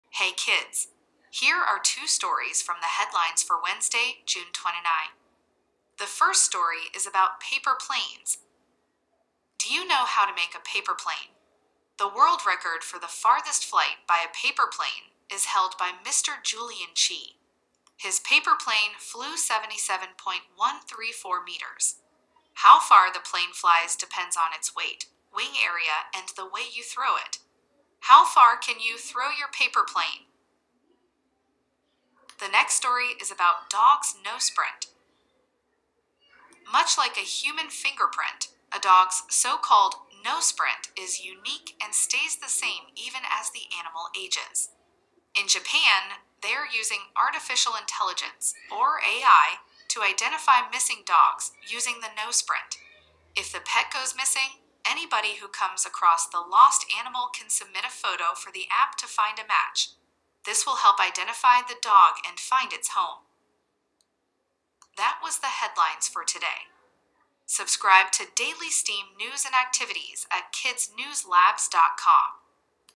Paper planes and Dog’s noseprint , Headlines June 29.
2 STEAM stories from the news for June 29, 2022. The first is about paper planes and second is about dog’s noseprint.